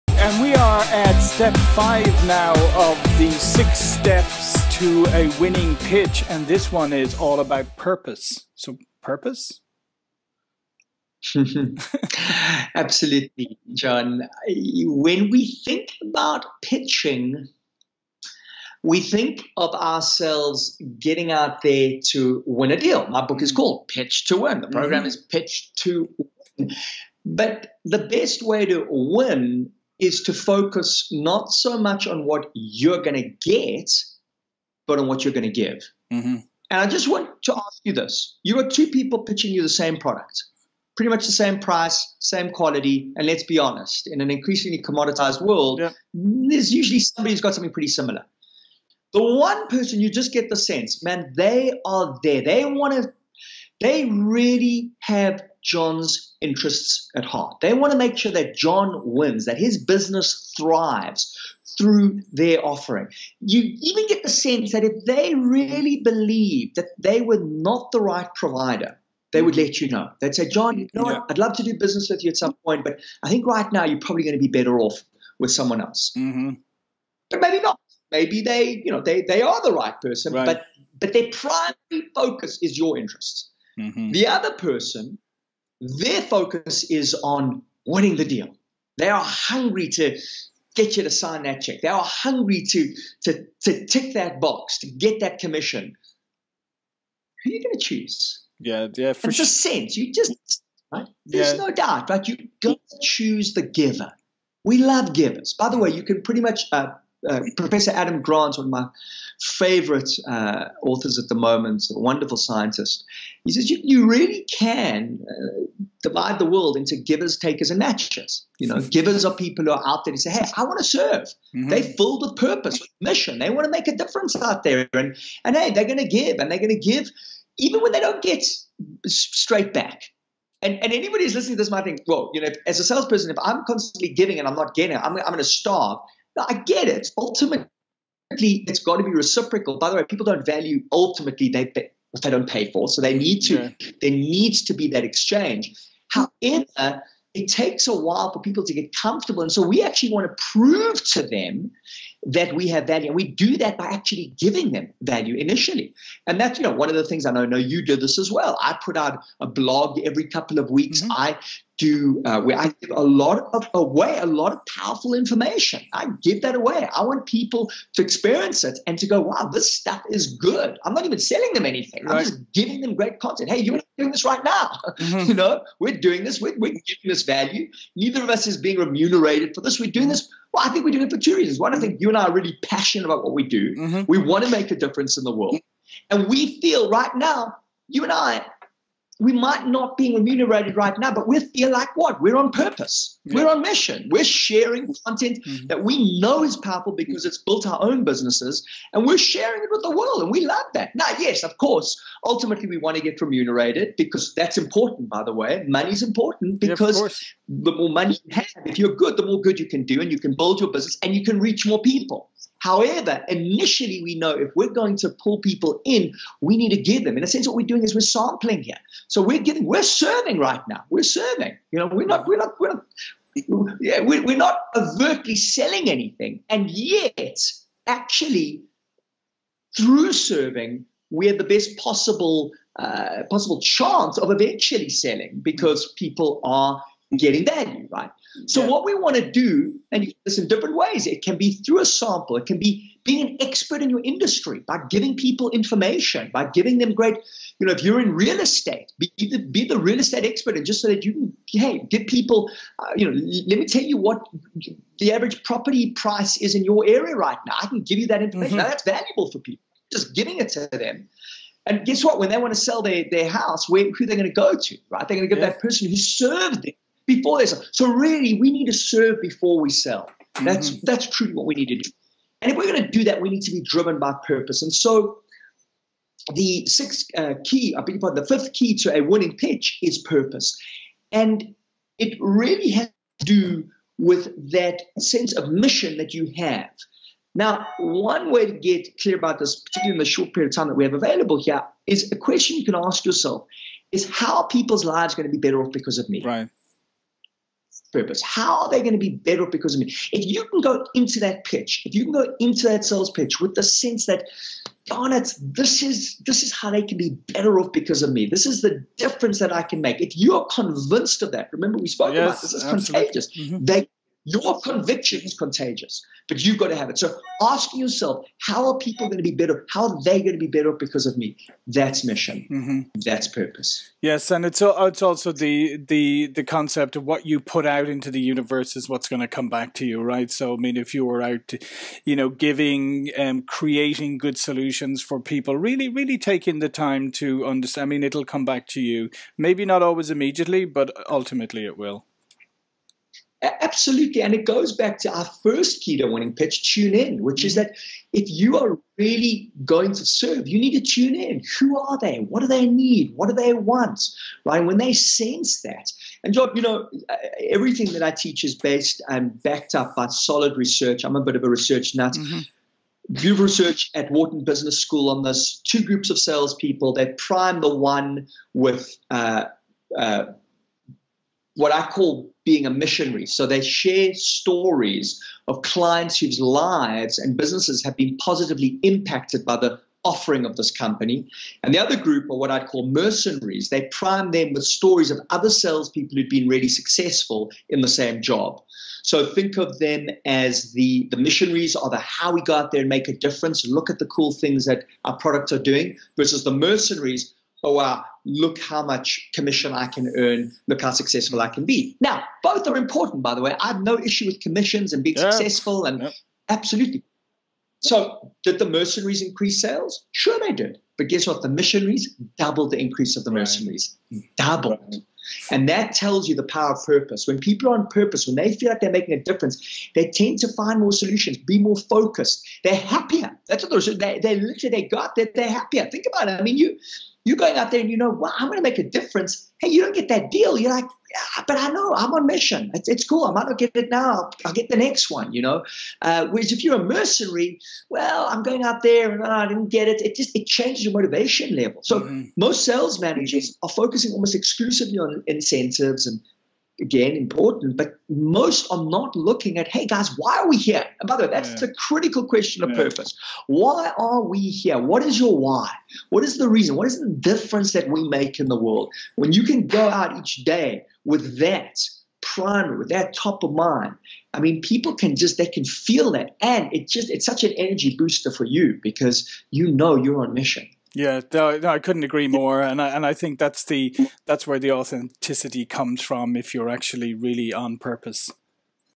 Pitch To Win with Justin Cohen: Step 5 Pupose | Sales Expert Insights Series
Justin Cohen New Book: Pitch to Win 6 Set Formula “Purpose”
Have you been following along with Justin Cohen and John Golden as they explore the six steps of pitching to win? Each week, SalesPOP! is bringing you a video detailing each step of the process, so by the end, you’ll be an expert on pitching to win. This video explores step five, purpose.